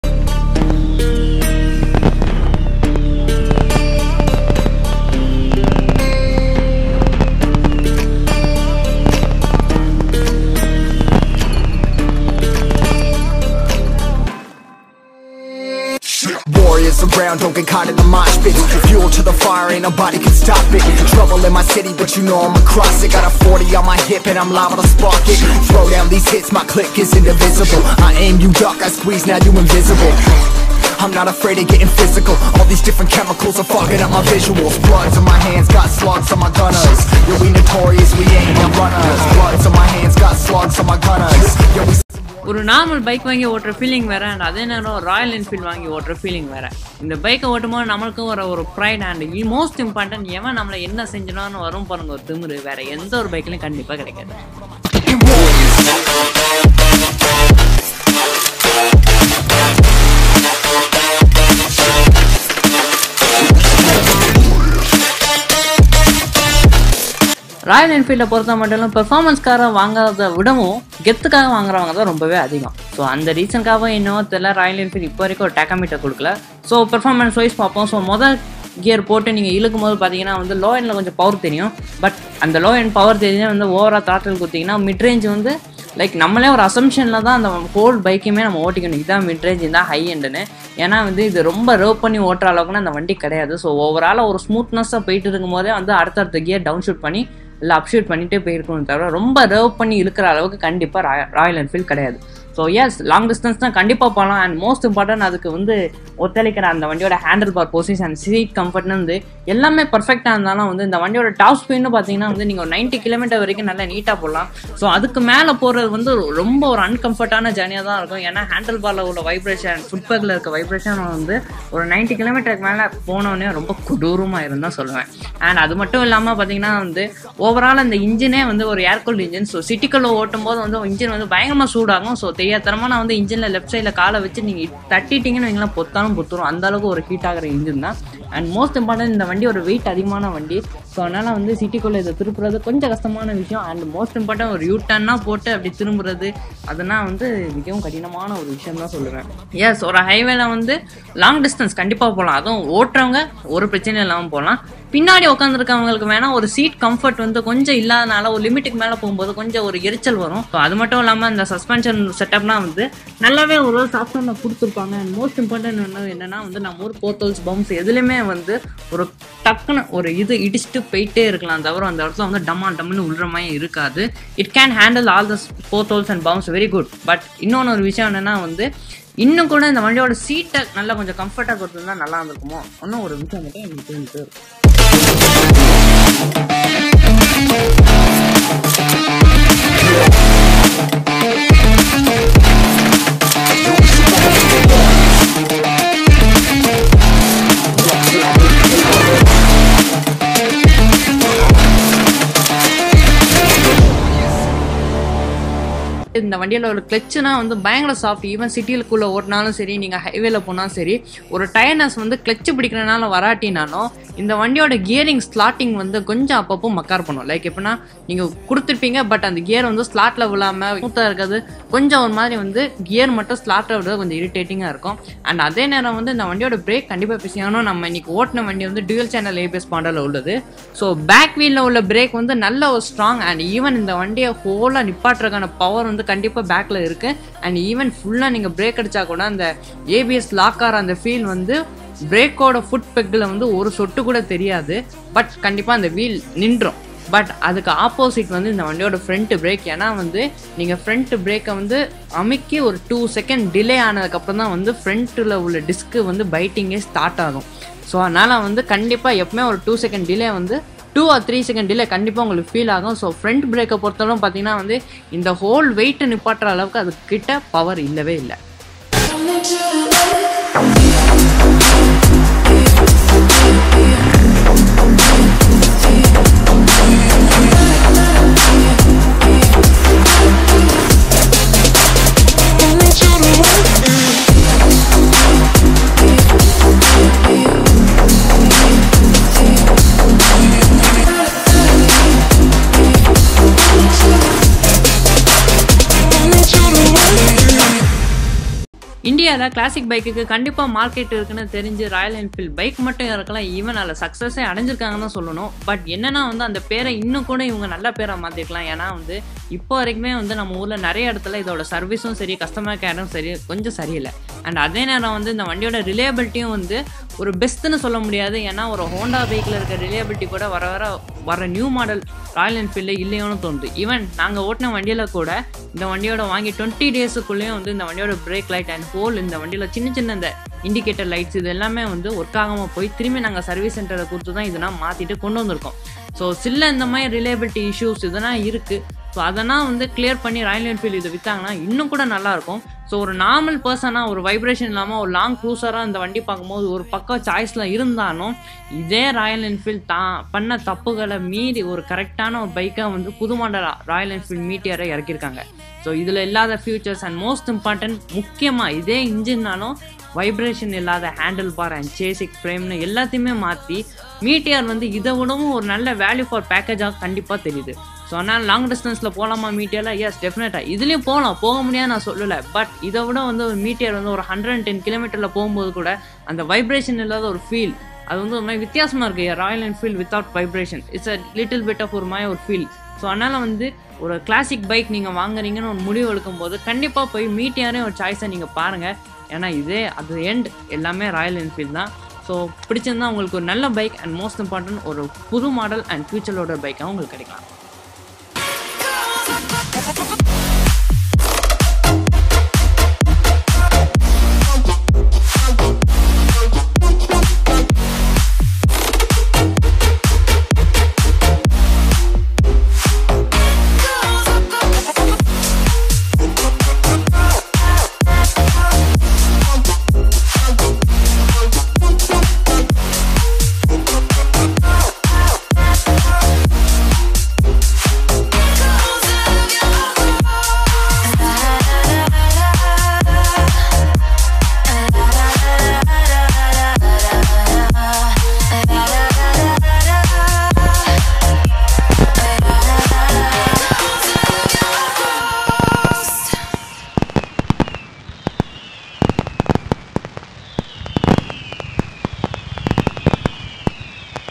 Royal Enfield Classic 350{Stealth Black}/CARS 2 Wiki தமிழ்
This video featuring the pros and cons of RE Classic 350.